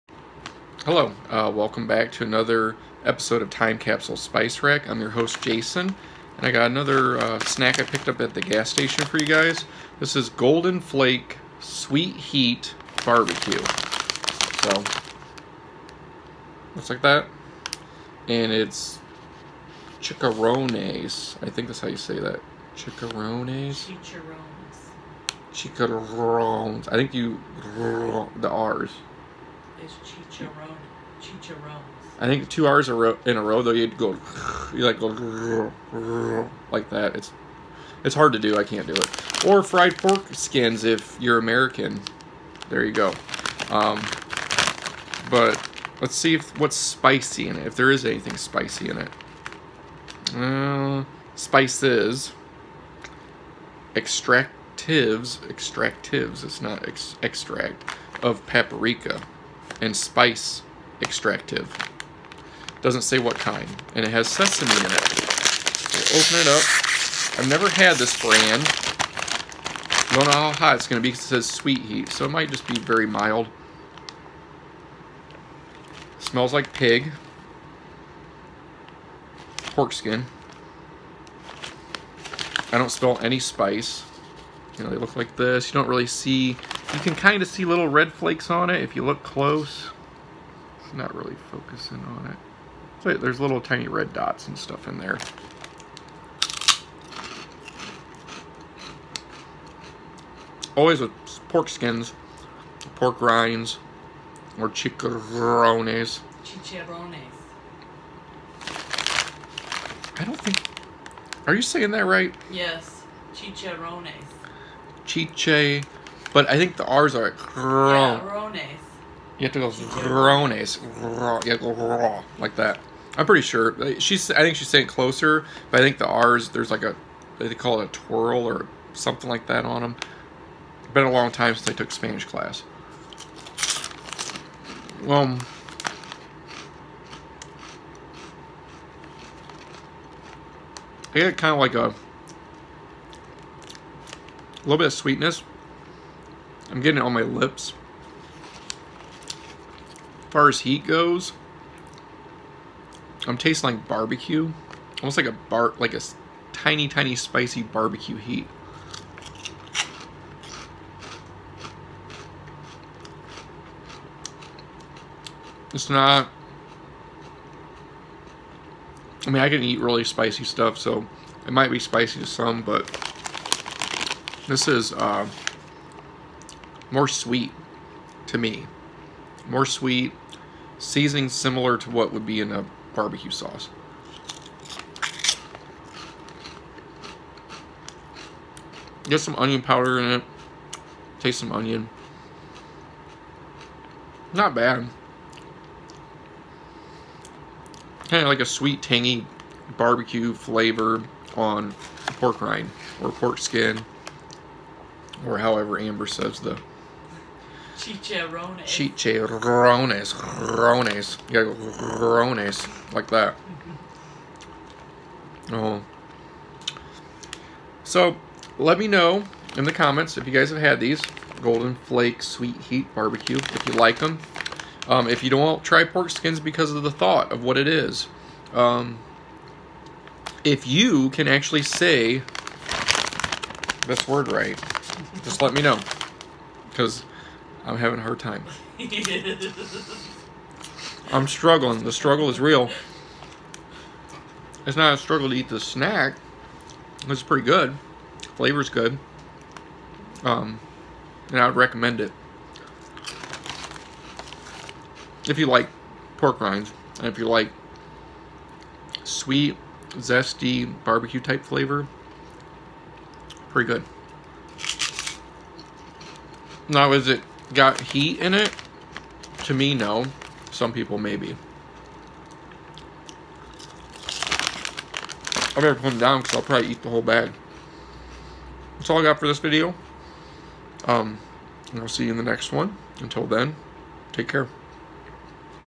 Sweet Heat Barbecue Fried Pork Skins or Rinds or Chicharrones by Golden Flake-TCSR#22
I try some good ole’ pork rinds or skins or chicharrones if you prefer by Golden Flake, Sweet Heat Barbecue.